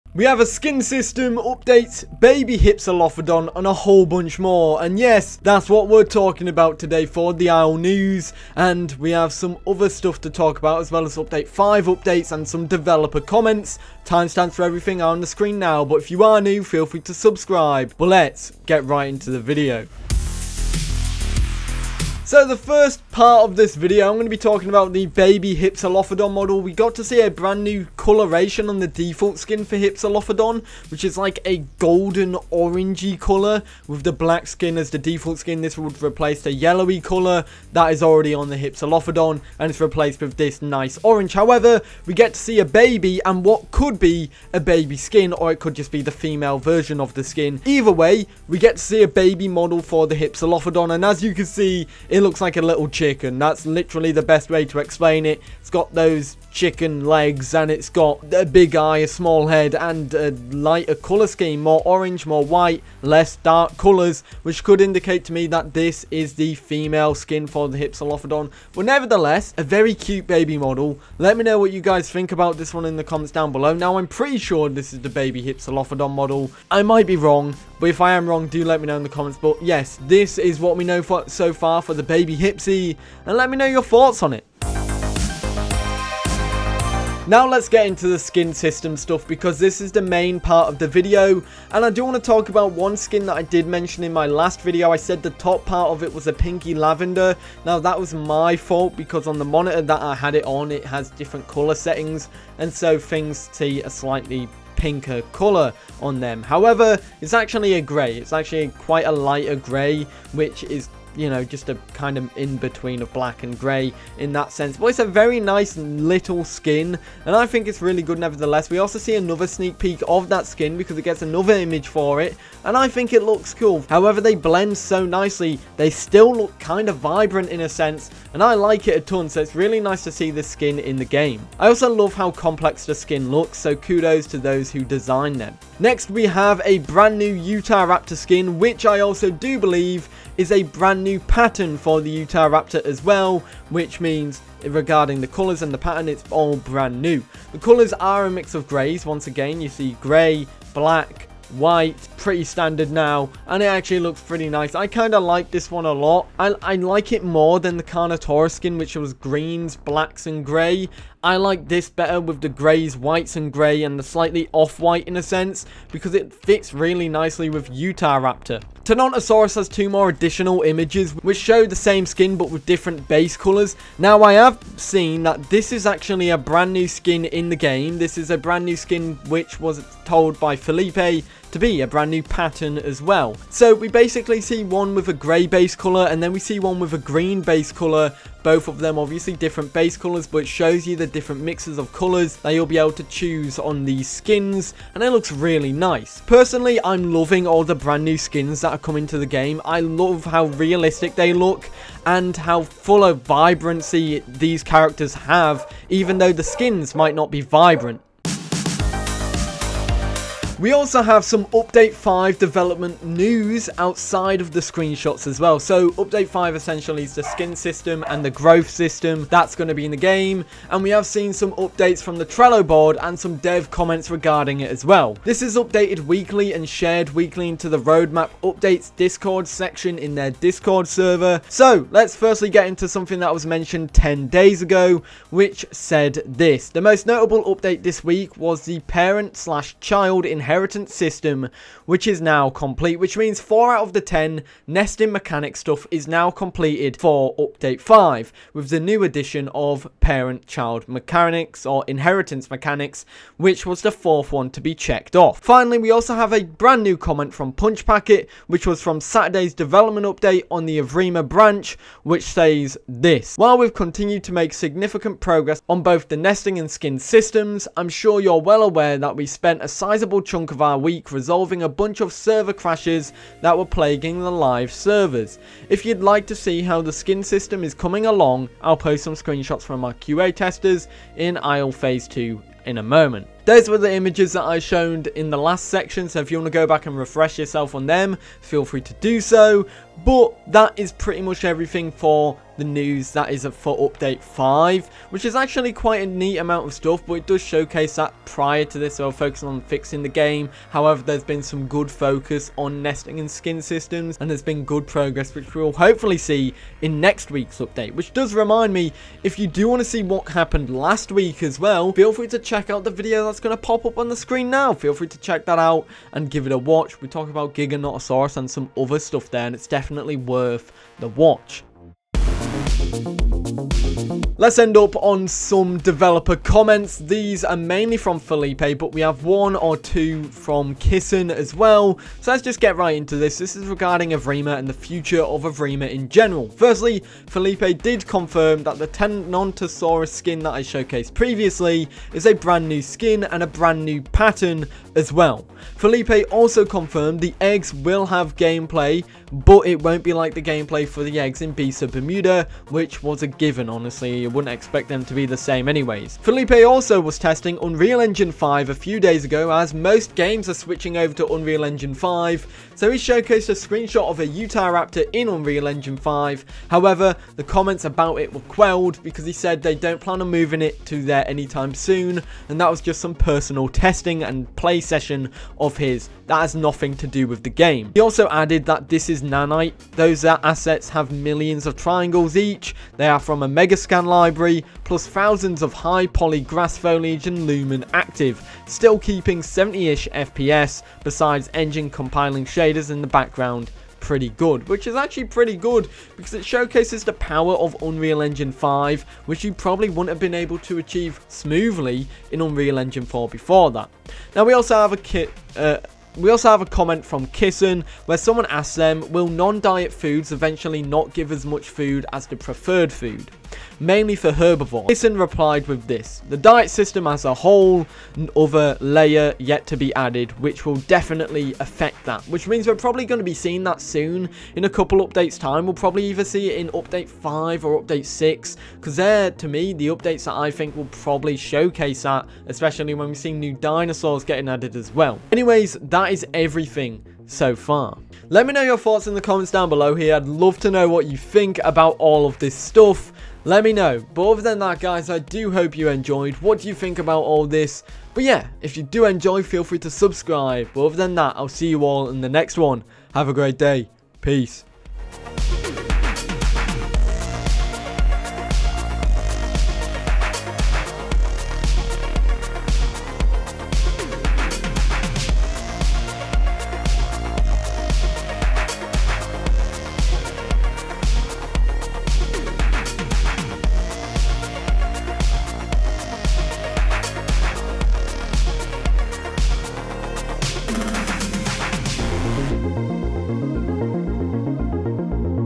THE SKIN SYSTEM WILL BE GREAT! | The Isle Evrima
The Skin system is heating up, Hypsi has a baby model, a huge load of development news for Update 5, theres a whole load of new The Isle news to share today, and so there is quite a bit to go through today! So, lets talk about it!

Timestamps:
00:00 - Intro
00:20 - Baby Hypsi
01:25 - Skin System
03:30 - Development Updates
05:25 - Developer Comments
07:20 - Outro

What is your opinion?

What is The Isle?
The Isle is the current top dog in terms of dinosaur survival games, its one of the biggest ones by far. With a whole host of dinosaurs and other prehistoric beasts on its Legacy branch.
The game is going through a total rework with its Evirma Update, an update that will change the whole dynamic of the game.